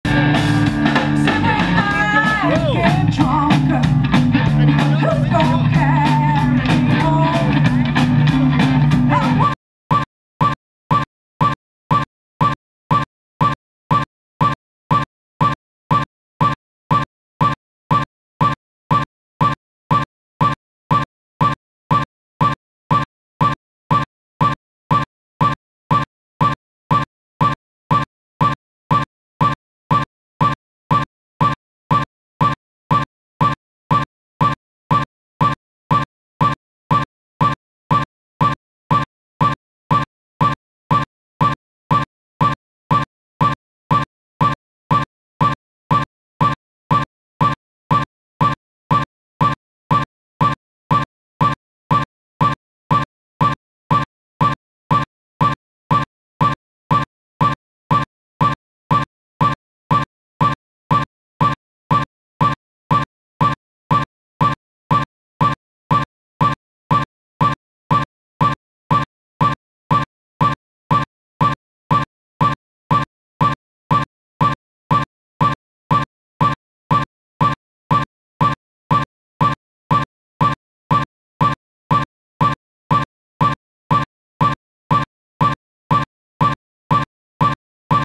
Micky Moody Band
Toneranger @ Eydon Beer Festival 21st June 2014, local beers and ciders with the blues of the Micky Moody Band.

intended.